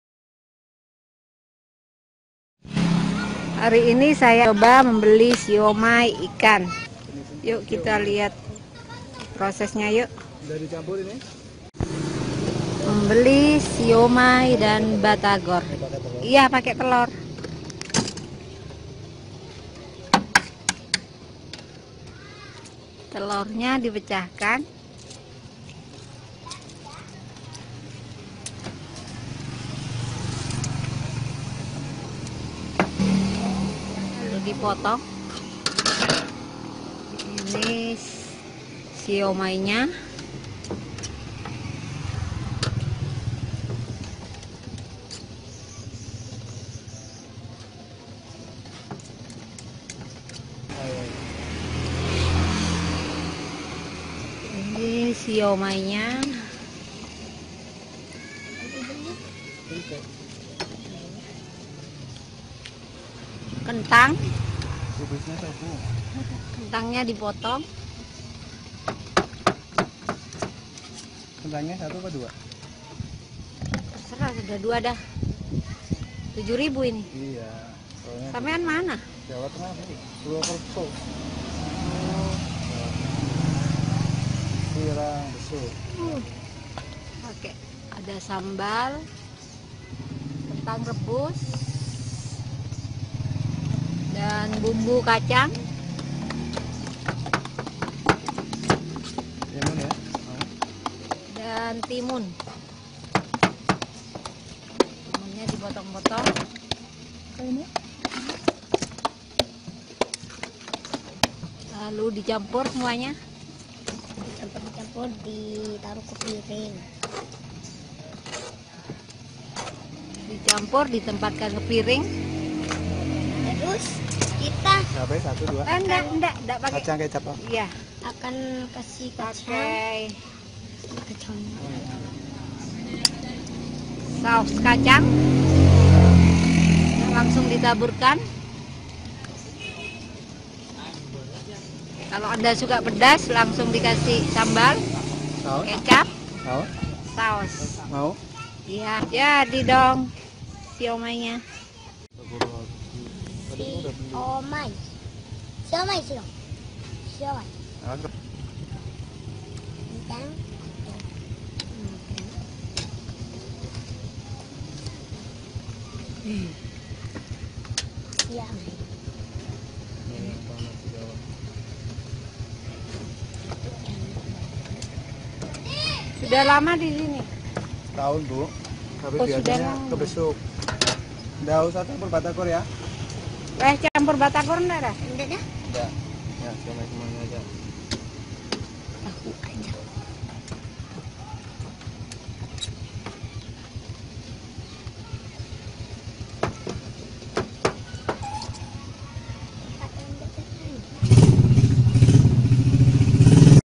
siomay bandung beli lagi yook..!
membeli siomay bandung rame-rame dan tahu apa saja bahan-bahan yang di gunakan dalam olahan siomay bandung.